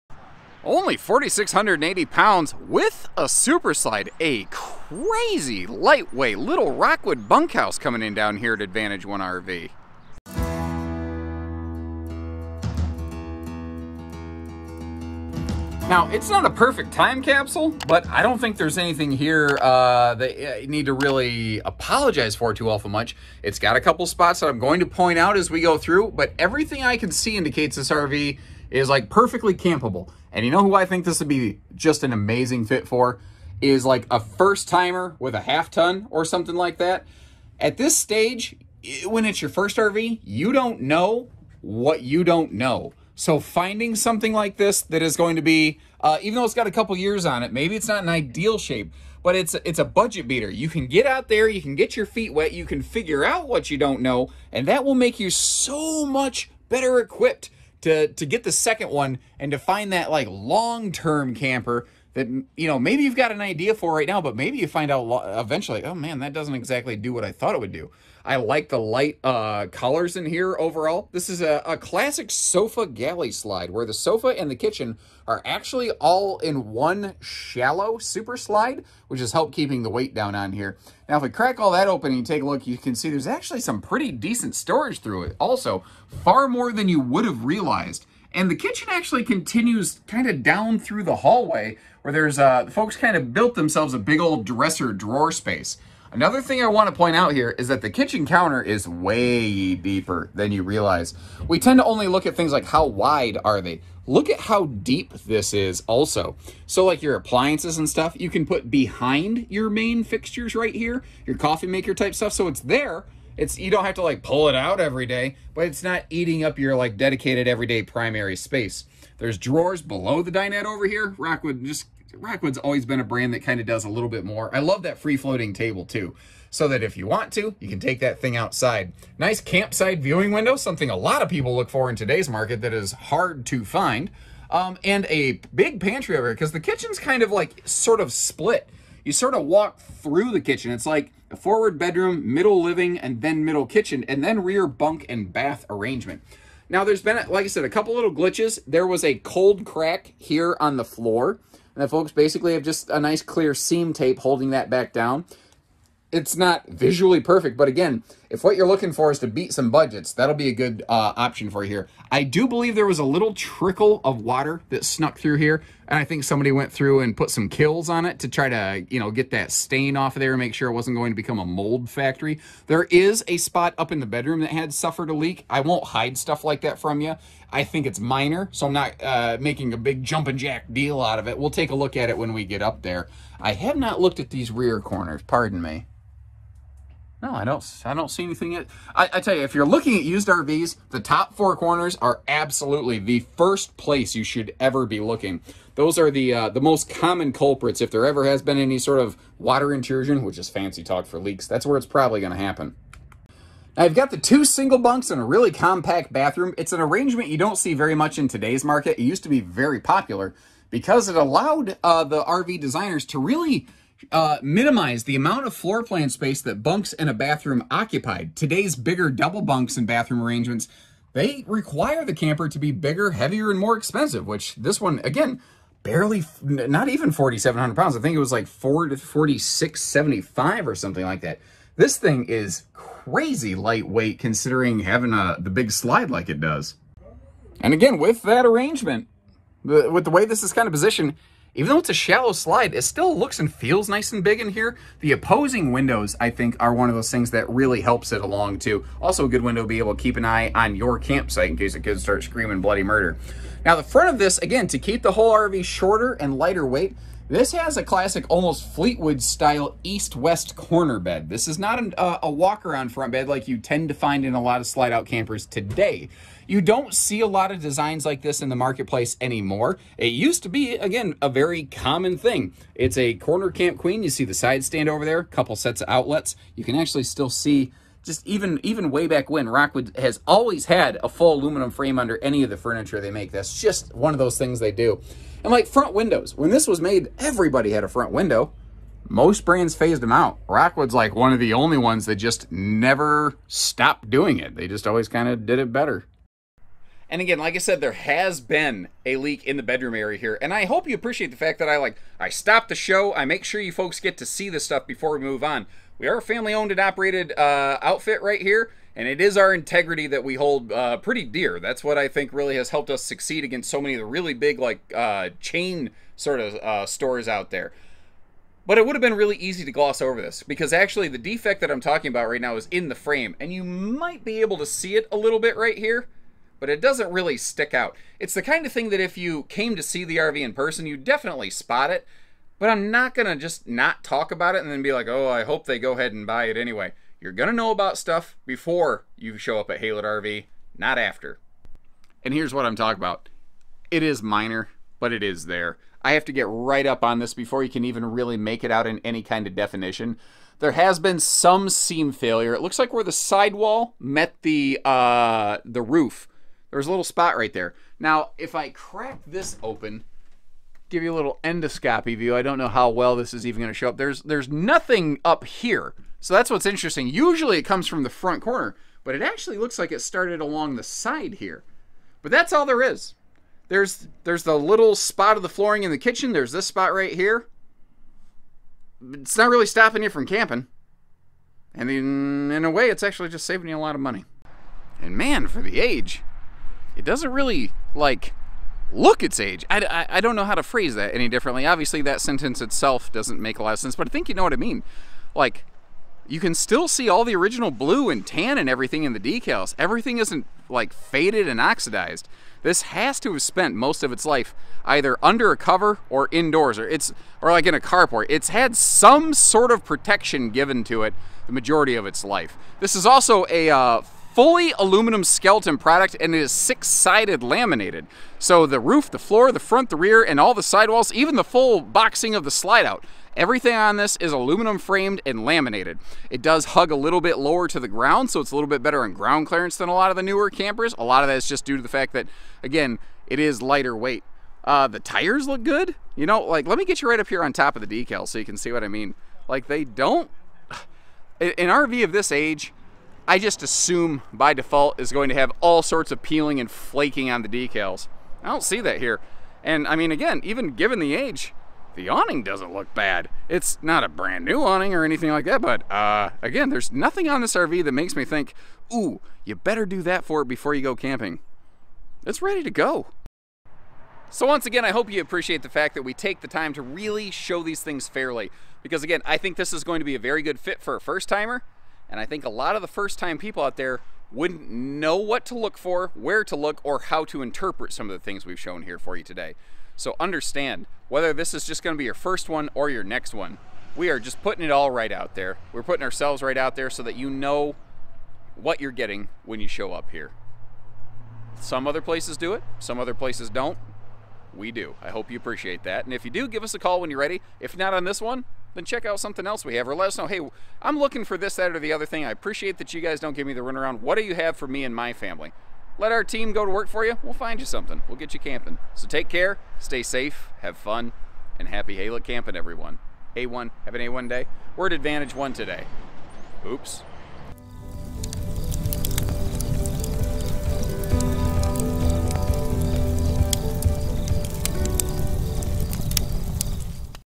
[$$$ BUDGET BEATER $$$] 2004 Rockwood 2601 (Sold)
2004 Forest River Rockwood Ultra Lite 2601 Bunkhouse Travel Trailer

Only 4,680lbs Empty Weight!
Length 29'3"
Width 8'0"
Height 9'4"
Ceilign 6'6"
Cargo 1,833 lbs.
Max Wgt 6,407 lbs.
Hitch 505 lbs.

We are Haylett Auto & RV of Coldwater Michigan! Family Owned & Operated Since 1989 and Going Strong!

Josh's Pokemon GO Trainer Code: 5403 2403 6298 (LOL)